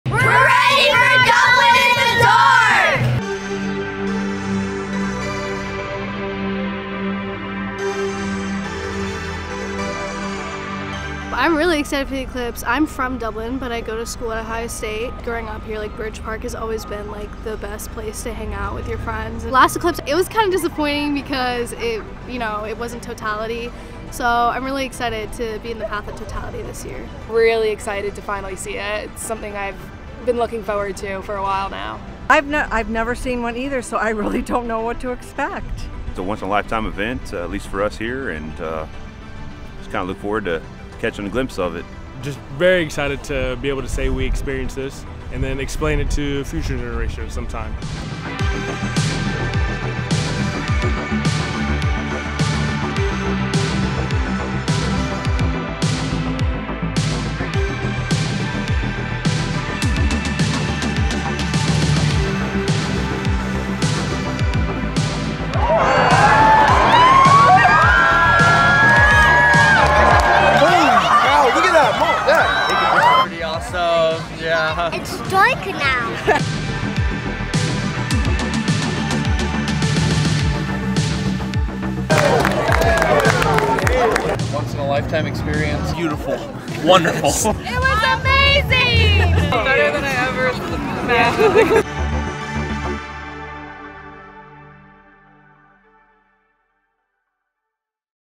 Dublin in the Dark – Total Solar Eclipse Recap
The Total Solar Eclipse was a once-in-a-lifetime spectacle. Thank you to all who joined us at Riverside Crossing Park and Coffman Park for our Dublin in the Dark party. Check out the City of Dublin's website for photos and video from this stellar event!